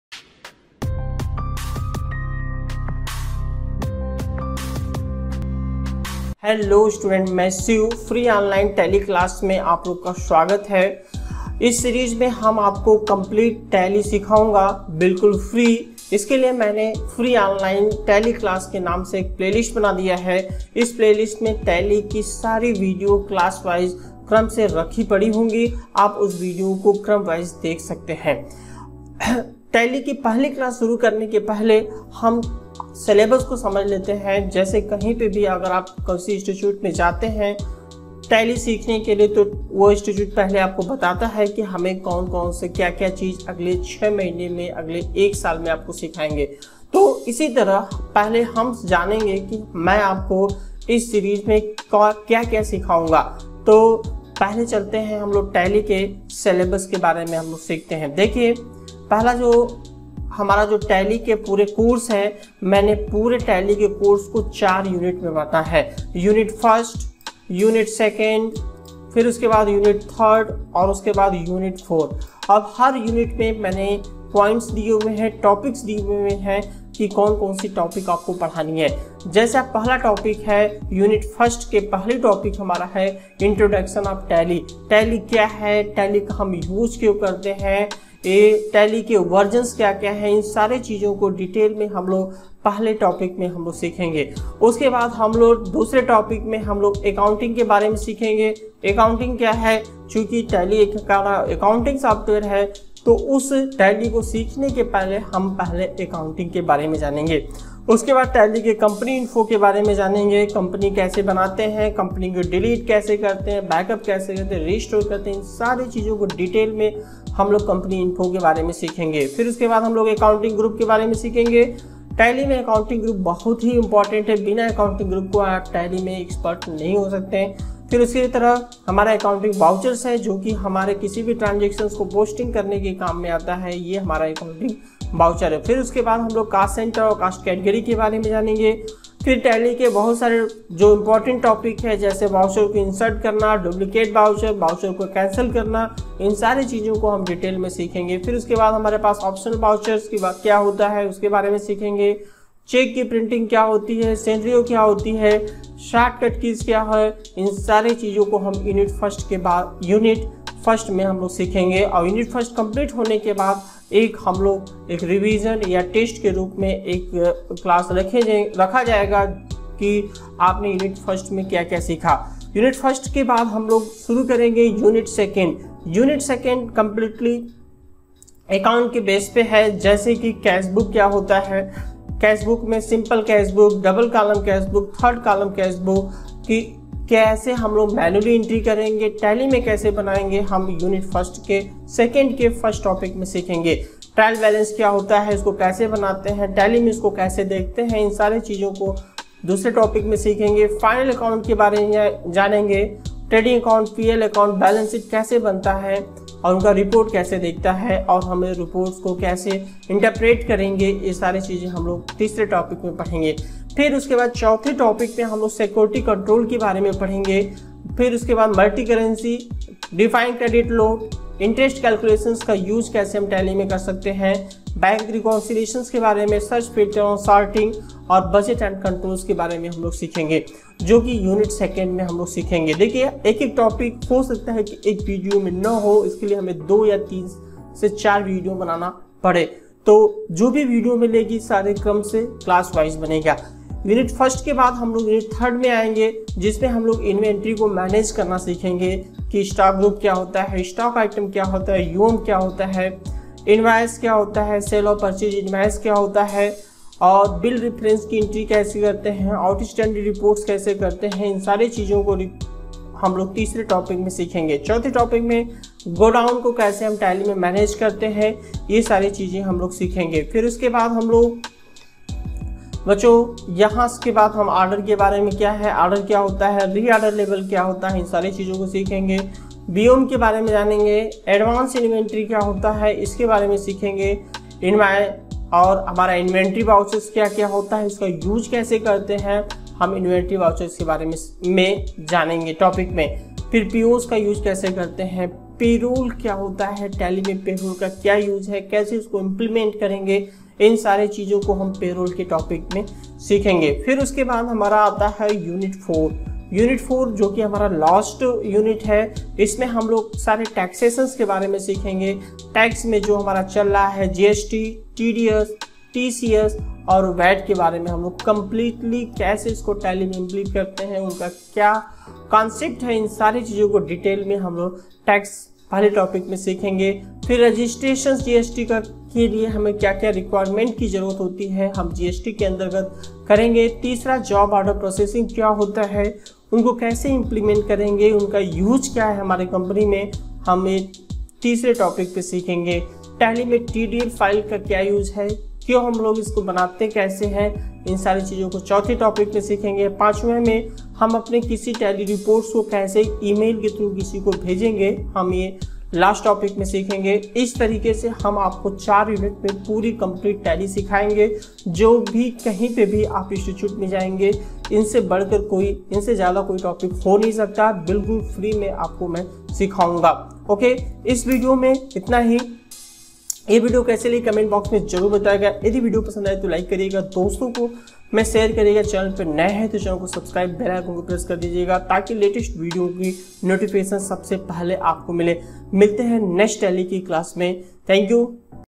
1:- Free online Tally class | Tally Syllabus | Class- 1 of tally
Hello Student,
           In this video ,I shall explain about tally syllabus .this video is the first class of free online tally class.